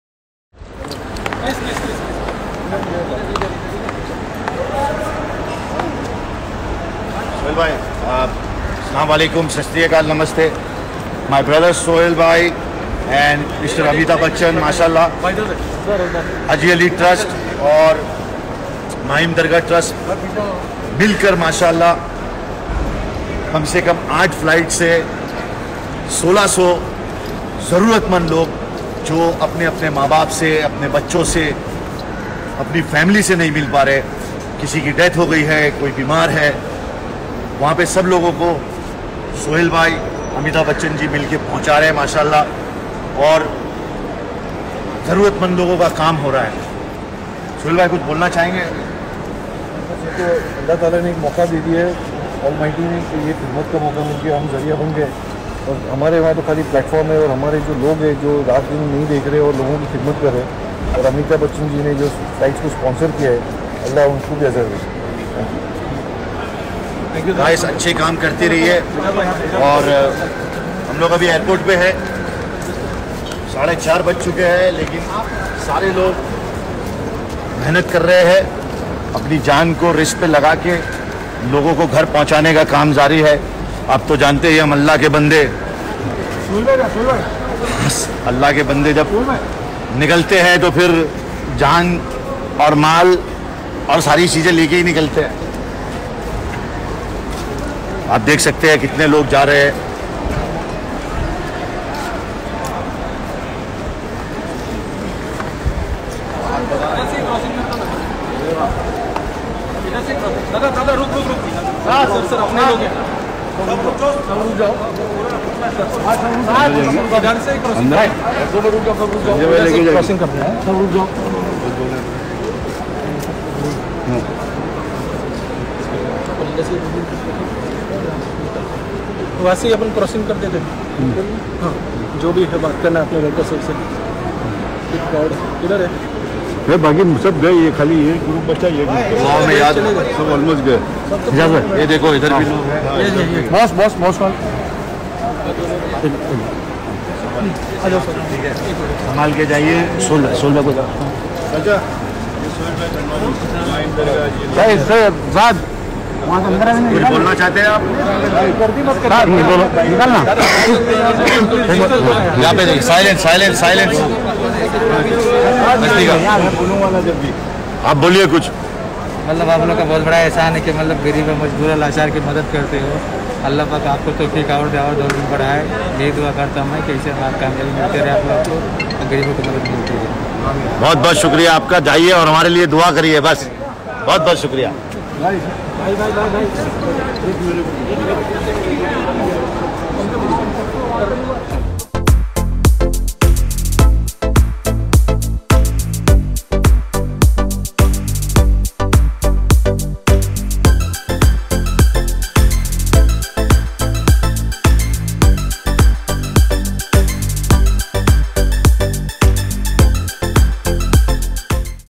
Ajaz Khan Help People Reach Their Home Through Airways
Ajaz Khan is yet again helping poor migrants who were unable to reach their homes, to reach their families safely amid coronavirus lockdown, apart from this Ajaz has been helping poor people by extending support by distributing food and essential items.